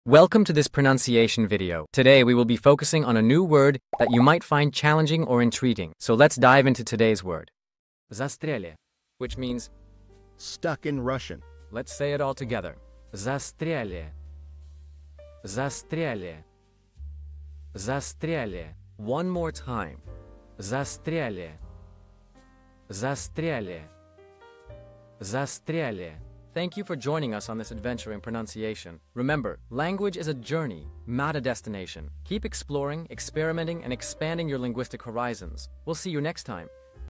How to pronounce Zastrjali | Застряли (Stuck in Russian)
Master the Pronunciation of 'Zastrjali | Застряли' - which means : Stuck in Russian 📝 with @PronunciationRussian 🎓🗣️ - [Your Guide to Russian]

Learn to pronounce 'Zastrjali' perfectly in Russian with our easy and engaging tutorial by @PronunciationRussian! 📖📽️ In this comprehensive video, we provide you with native speaker examples 🗣️👂 and phonetic breakdowns 🎵 to ensure you get the pronunciation just right.